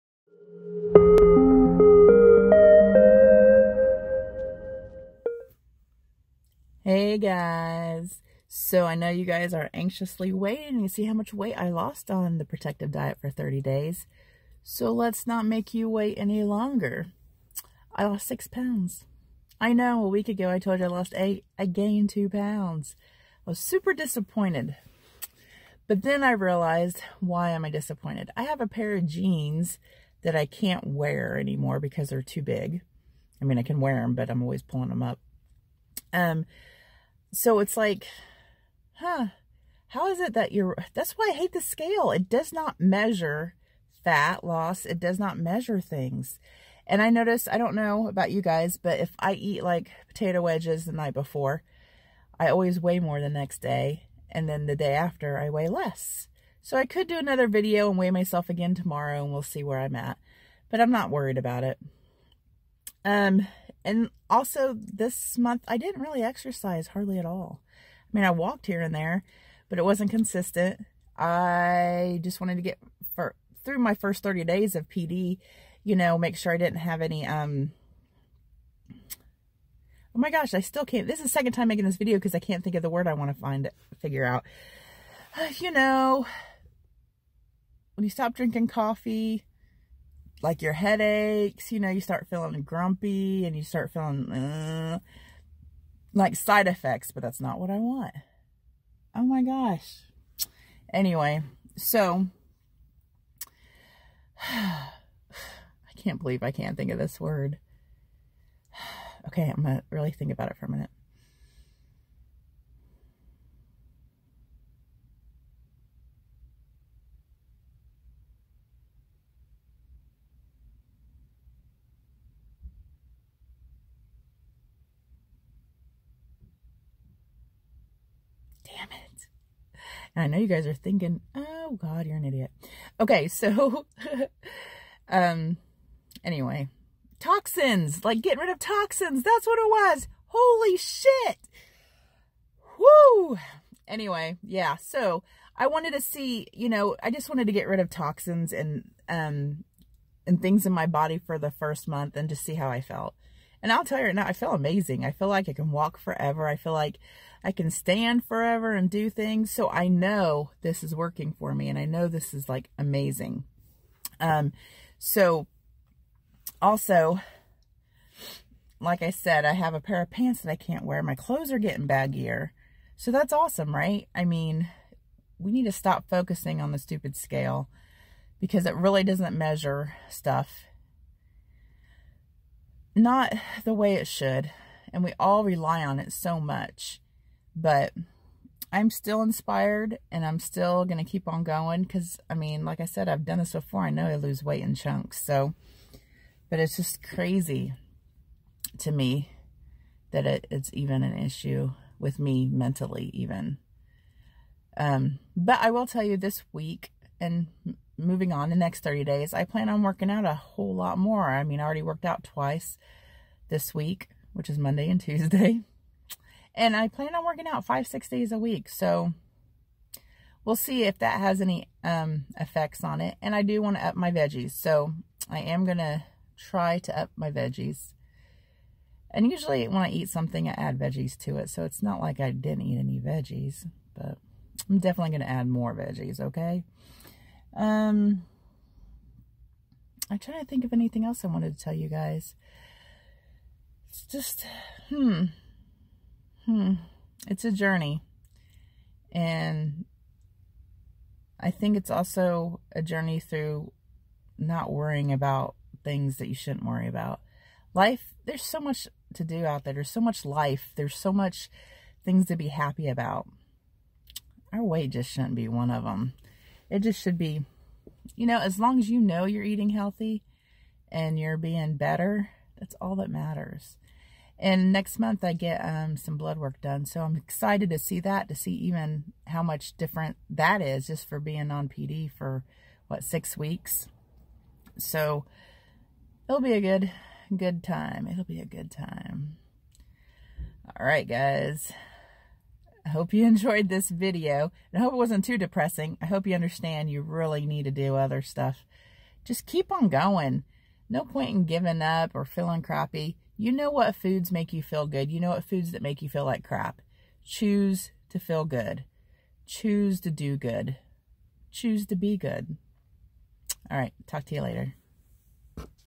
30 DAYS ON PROTECTIVE DIET!! MY RESULTS!!
30 days without meat, dairy, sugar, oil, nuts or processed foods!